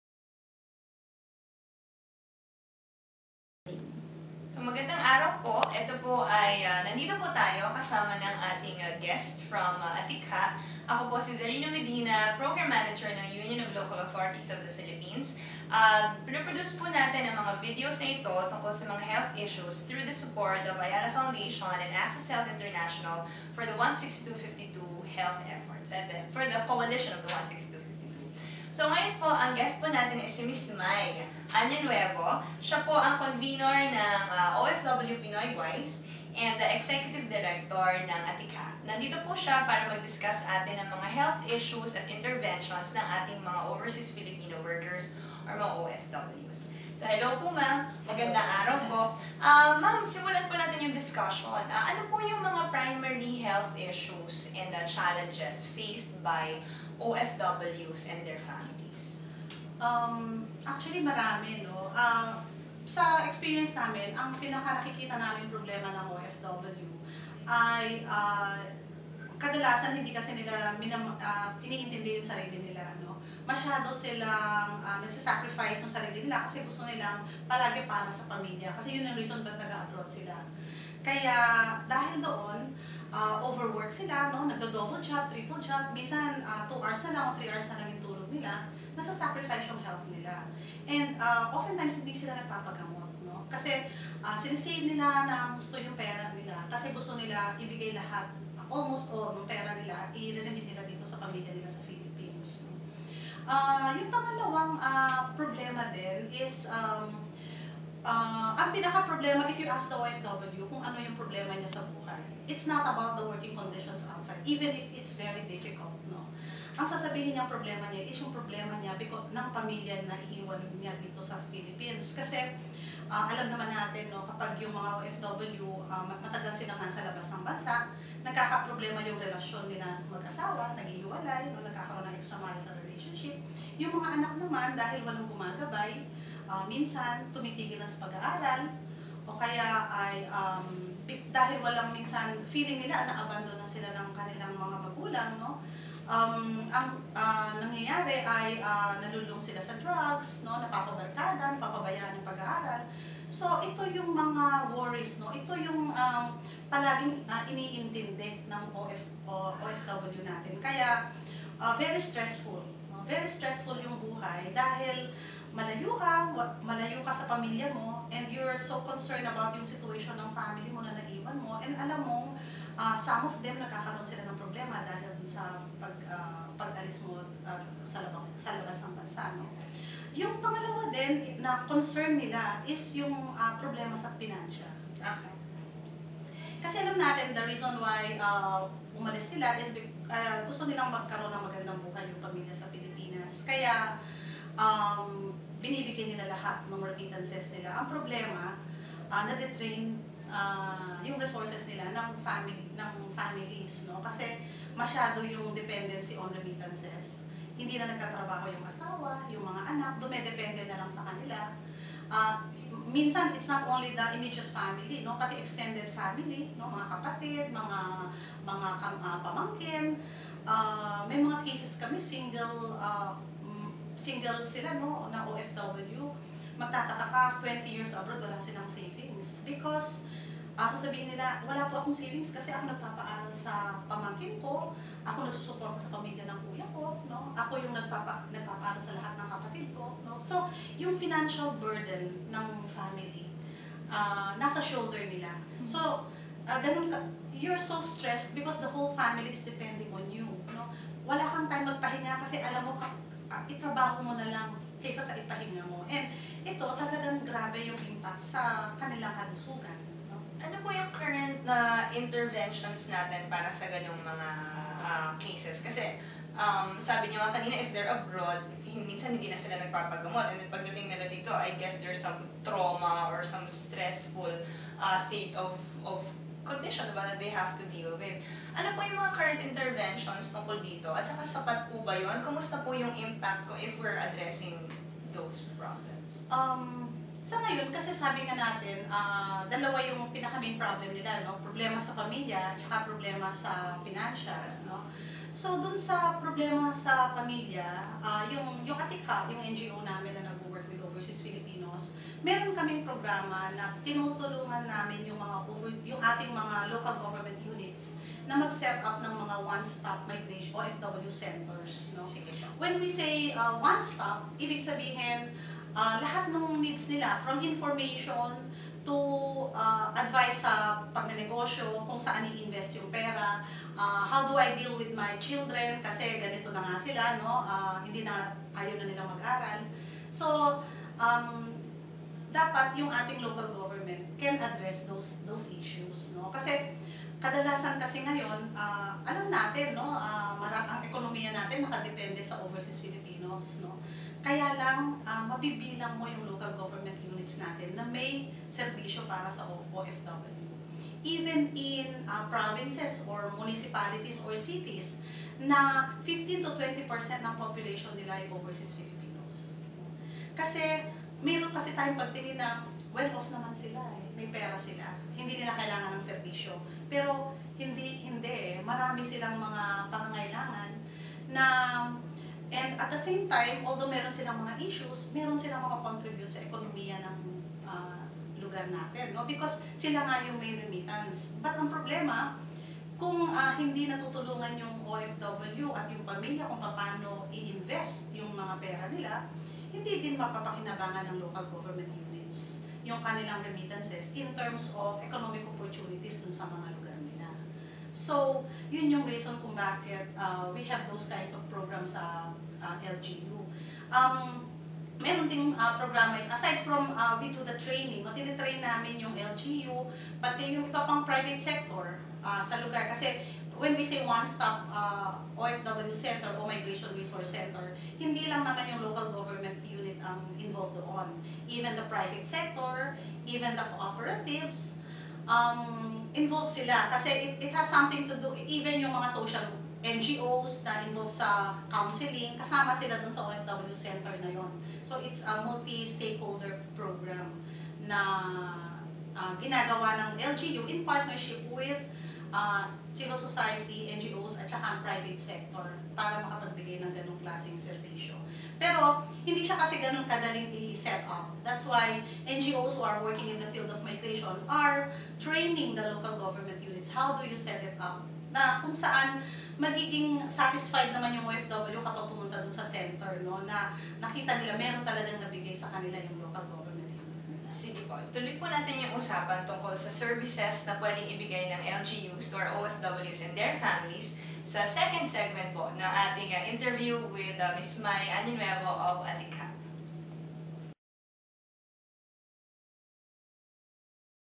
Health PolicyTube on Health Concerns of Overseas Filipino Workers (OFWs) (Part 1 of 3)
A health policy discussion series of the Union of Local Authorities of the Philippines (ULAP) filmed with the Ayala Foundation/ACCESS Health International, Philippines for the 162to52 Coalition on Health Concerns of Overseas Filipino Workers (OFWs)

Moderator:
Czarina Medina, Program Manager, Union of Local Authorities of the Philippines

Resource Speaker:
Mai Anonuevo, Executive Director, Atikha